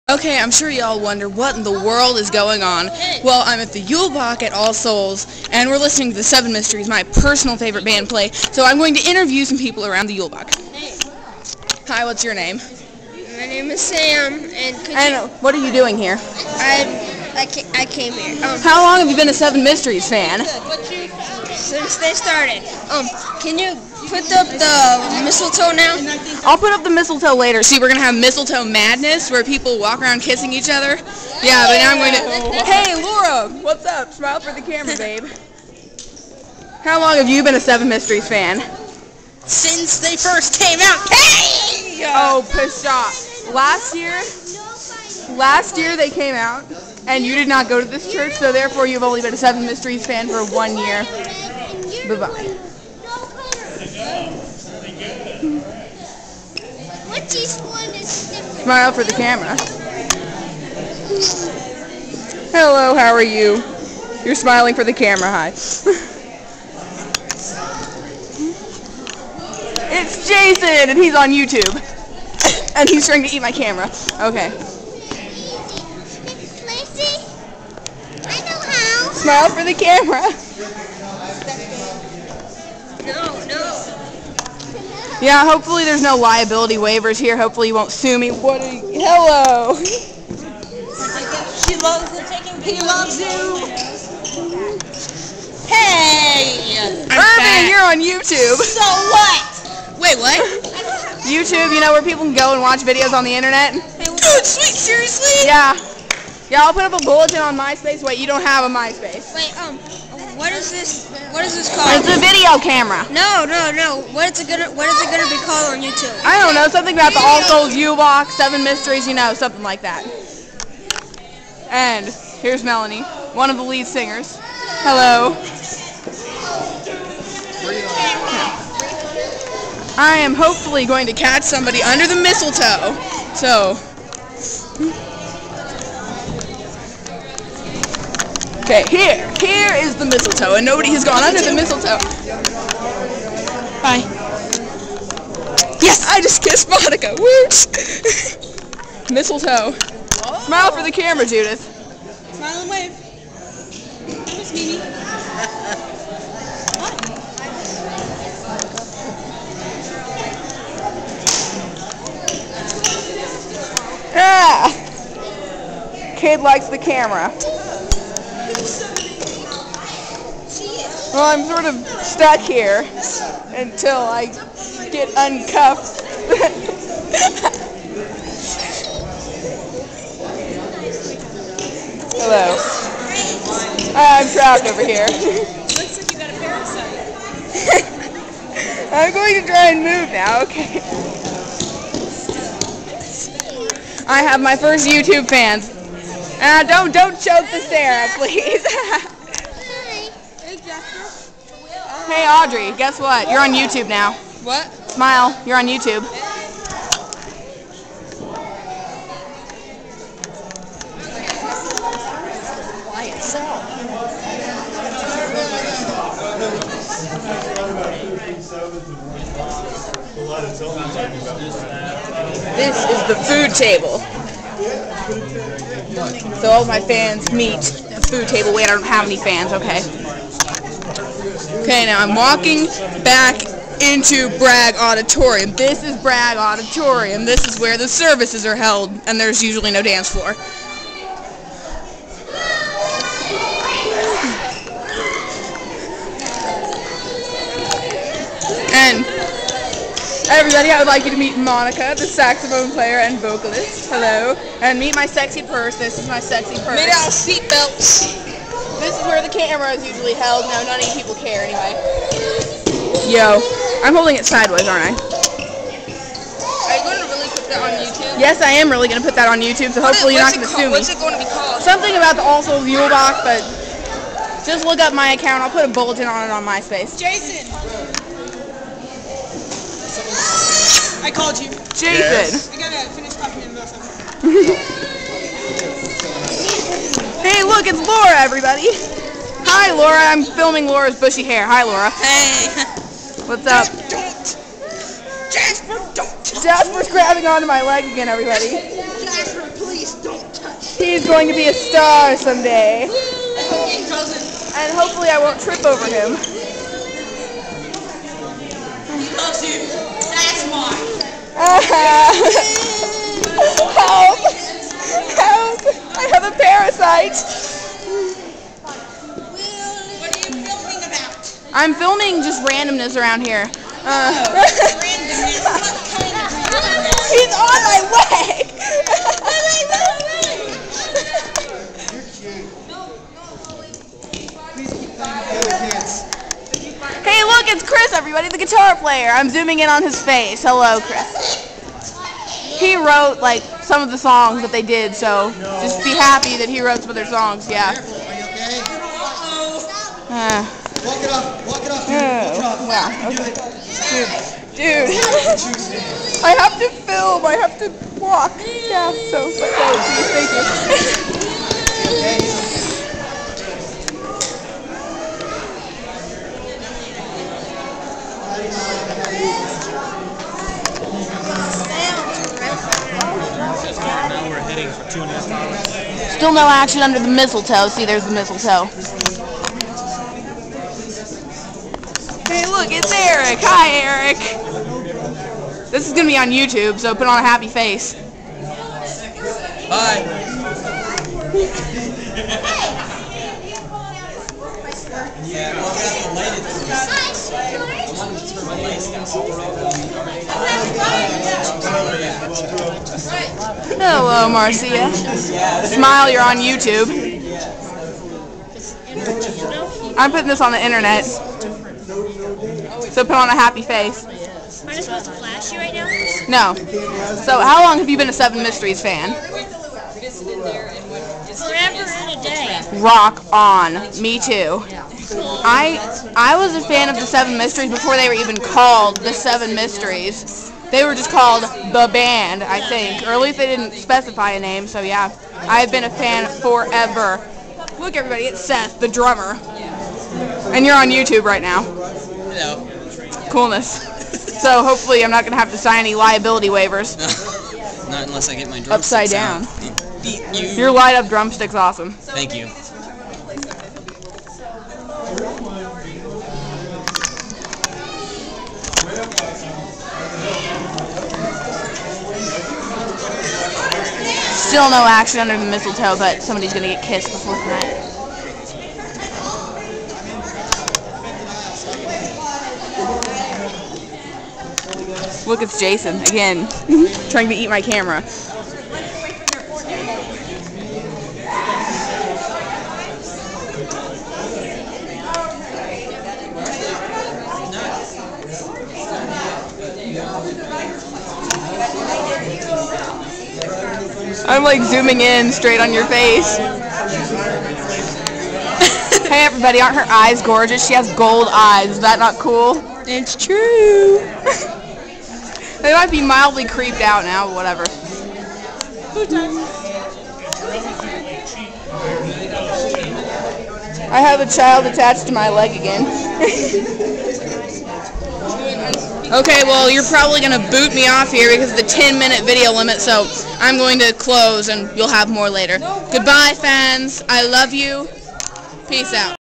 All Souls Yuleboeck-Interviewing party guests/band members
This video shows me interviewing many of the 7 Mysteries members/party guests at the all souls yuleboeck.